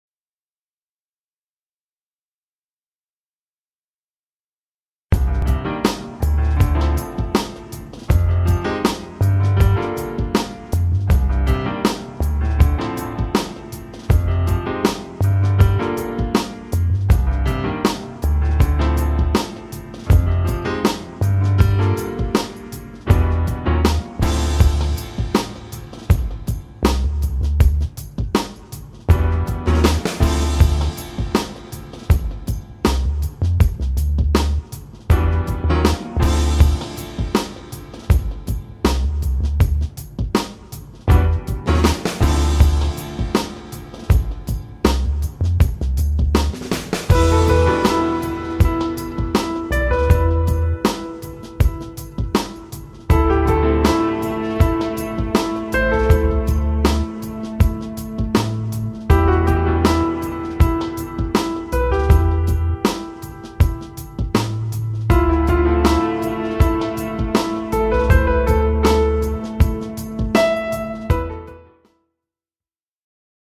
Fiat 500 occasion visible à Orthez présentée par Nissan orthez
Cette Fiat 500 occasion disponible à Orthez dans le 64 est mise en vente par Nissan orthez - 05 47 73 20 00 - tous les détails sont